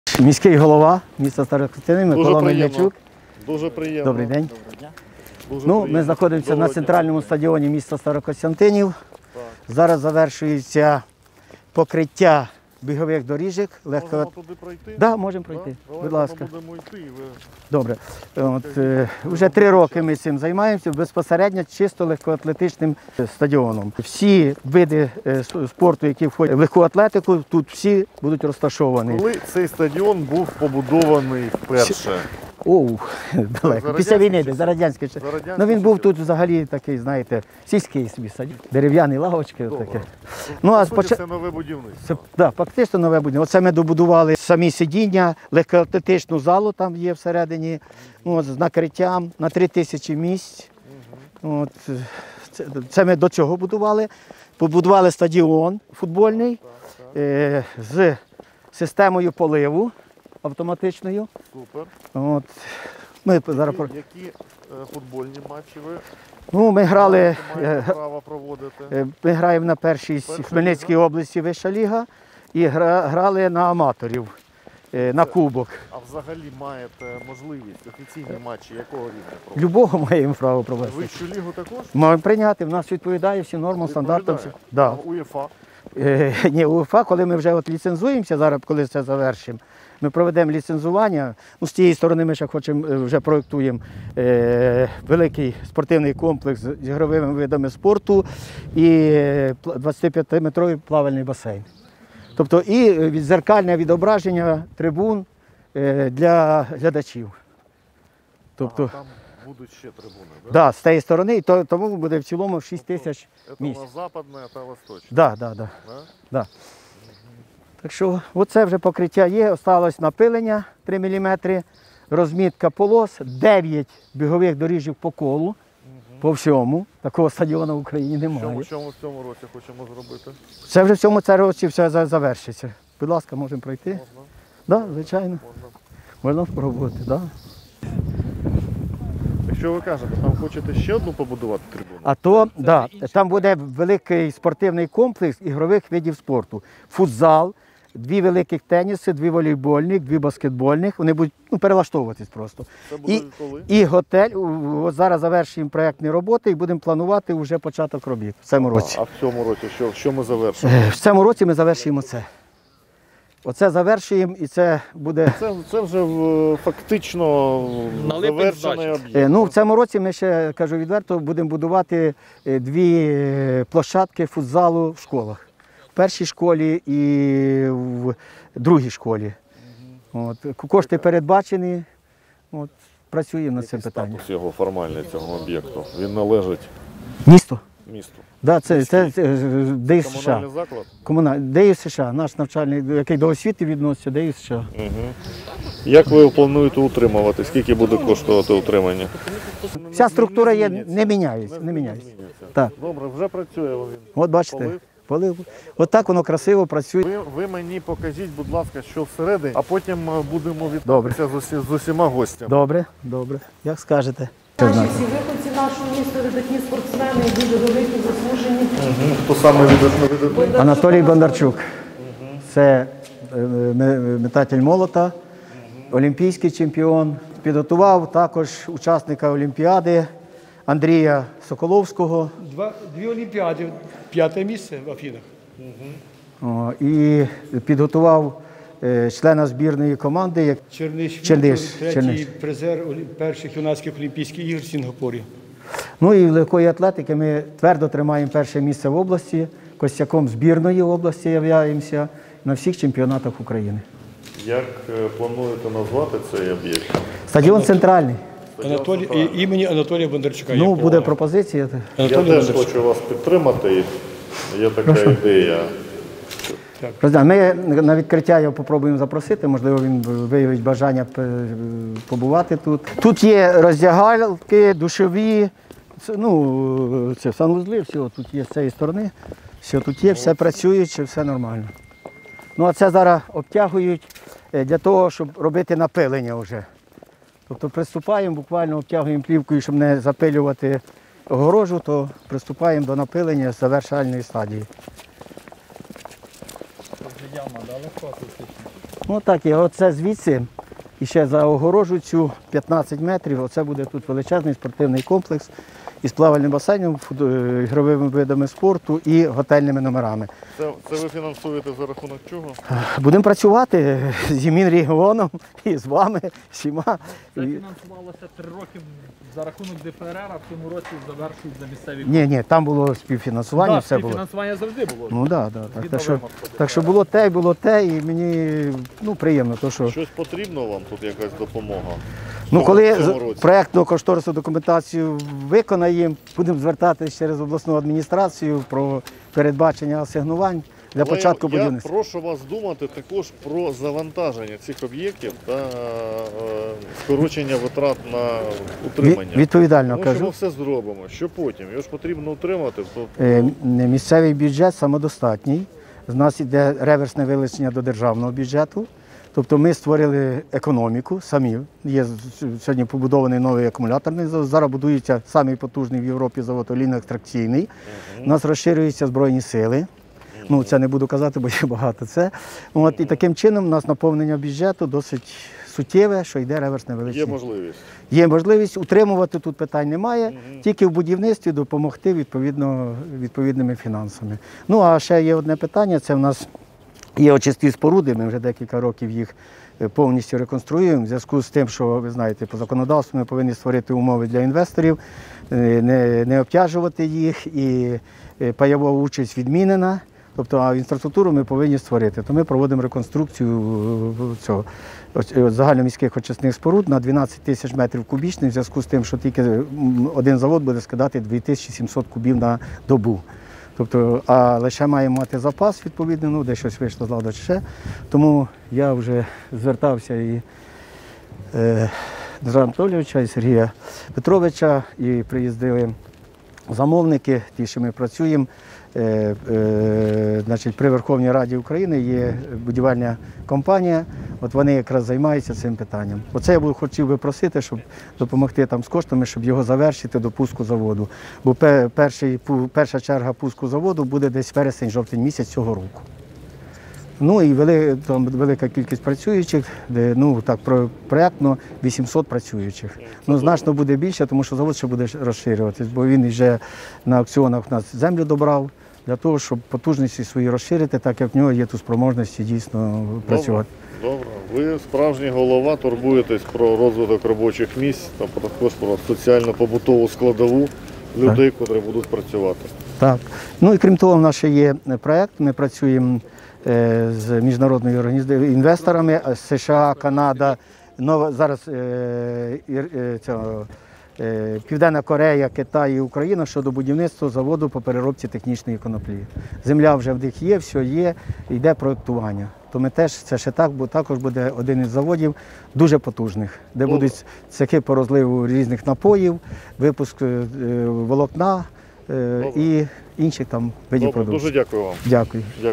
Робоча поїздка Міністра Олексія Чернишова до Старокостянтинова
21 травня 2020 року у Старокостянтинові побував Міністр розвитку громад і територій Олексій Чернишов разом із керівництвом Хмельницької обласної державної адміністрації під час робочої поїздки до Хмельницької області. Міський голова Микола Мельничук ознайомив міністра Олексія Чернишова, голову Хмельницької ОДА Дмитра Габінета, заступника голови Хмельницької ОДА Сергій Ткачука, директора департаменту розвитку громад, будівництва та житлово-комунального господарства Хмельницької ОДА Віру Бригадир із ходом будівництва стадіону "Центральний".